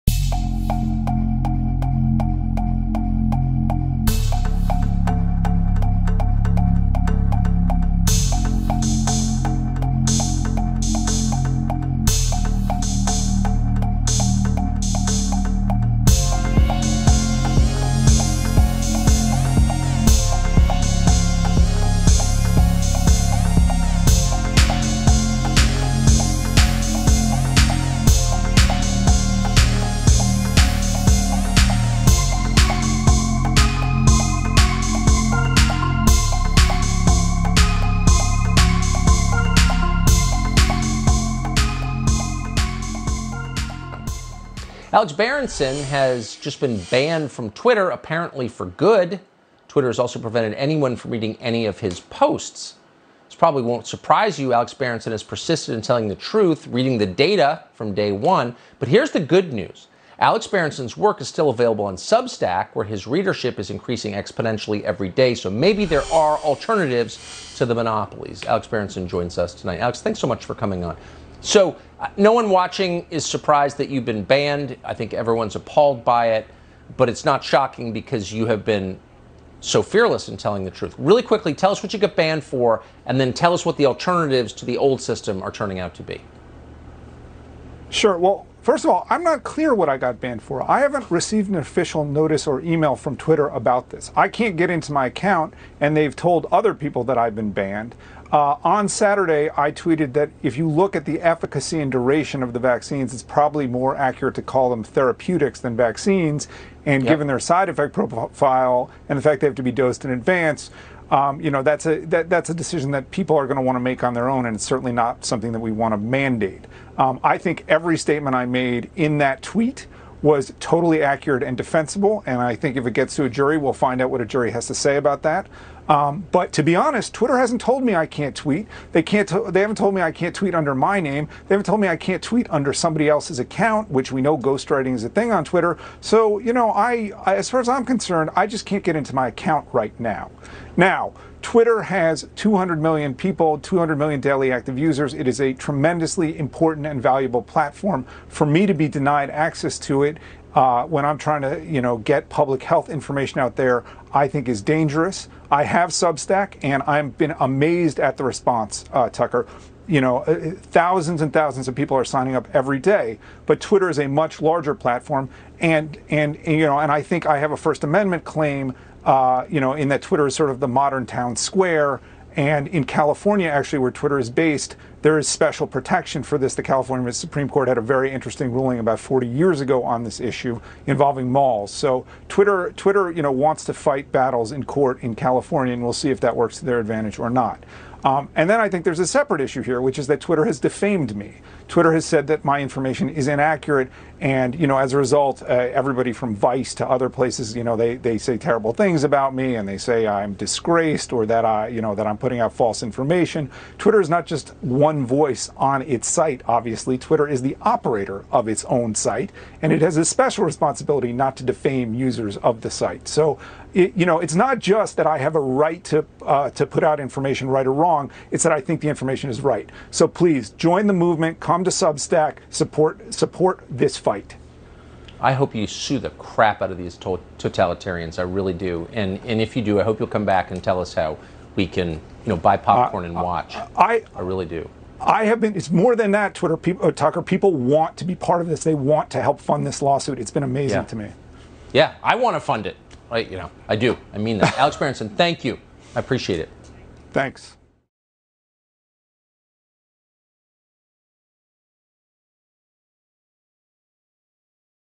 Alex Berenson banned from Twitter! Here is why and his reaction to it
Tucker Carlson hosted Alex Berenson who was banned from Twitter merely for telling people the truth about the vaccines, which isn't welcome by the Big tech monopolies.